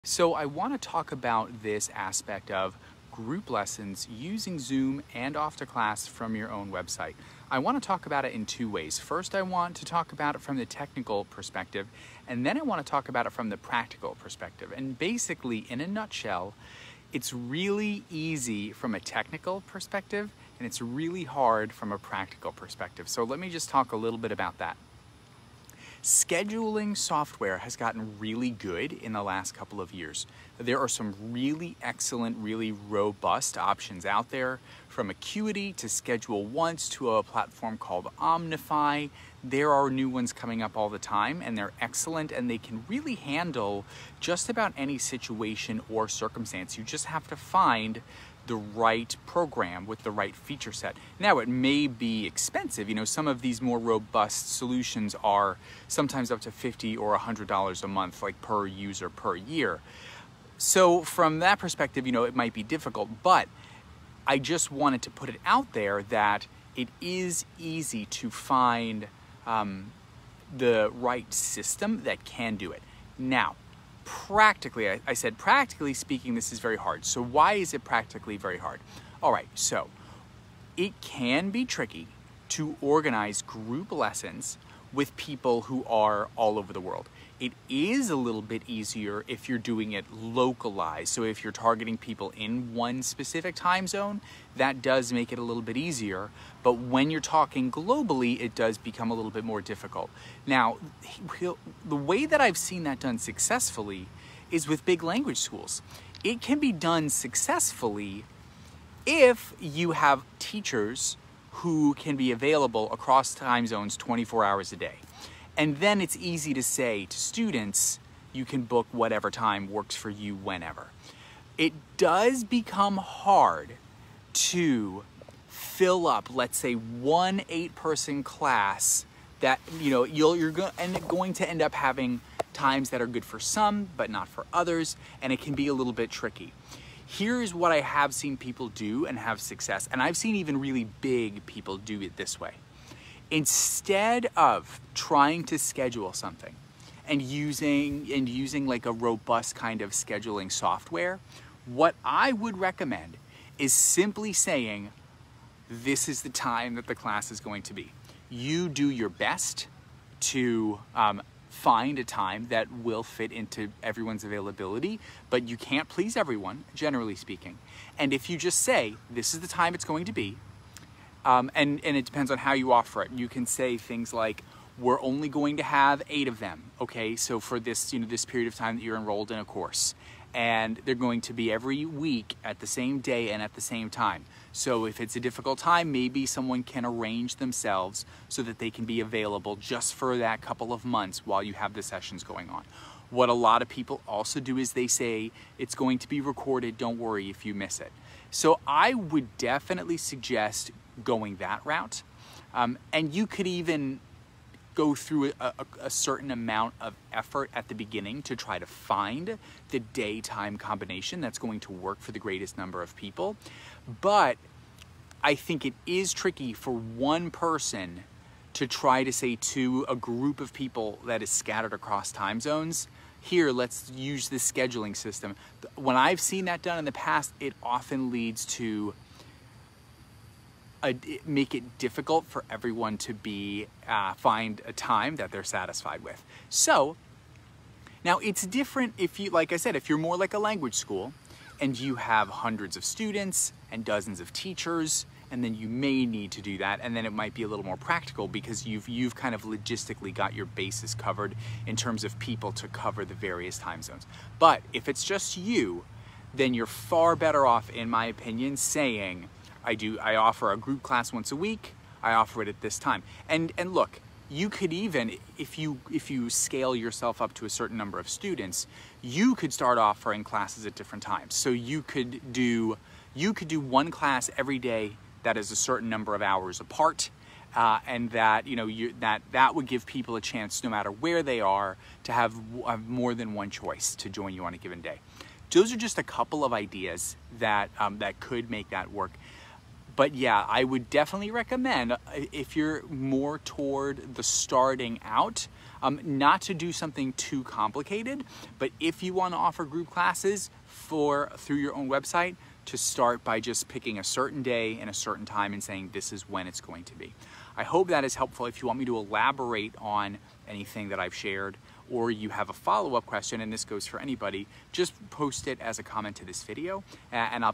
How to schedule students using Zoom and Off2Class
Do you use Zoom and Off2Class to teach group lessons?  Learn about the technical and practical perspectives you can use to implement Zoom and Off2Class effectively. This is a great video for teachers looking to learn how to schedule students as well!

Off2Class is an ESL toolkit designed to support teachers and schools. We help you deliver great ESL experiences to your students.